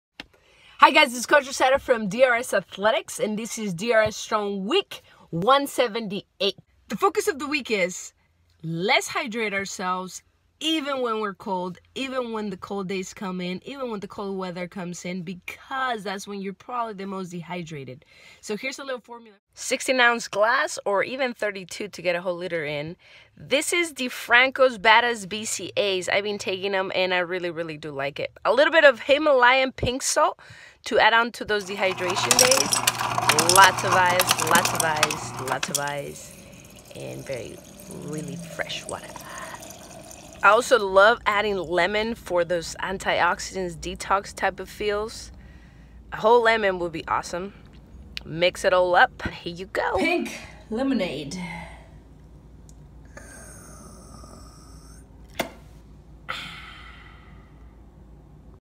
DRS Strong Week 178: LET’S HYDRATE!!
Check out yummy recipe for repleneshing liquids and keep ourselves hydrated even during cold/hot weather !